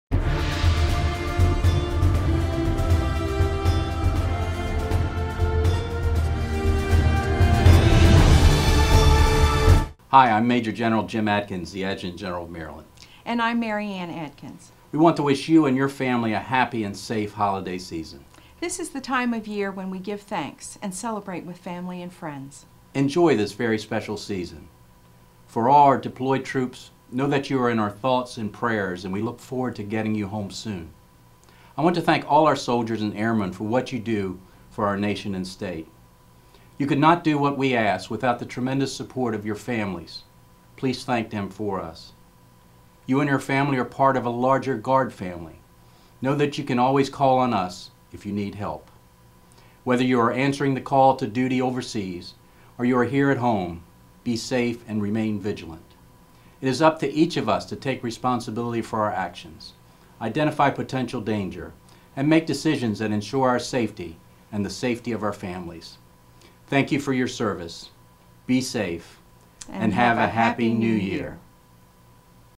Holiday Safety Message
Maj. Gen. James A. Adkins and Mrs. Mary Anne Adkins address members of the Maryland National Guard in this holiday safety message.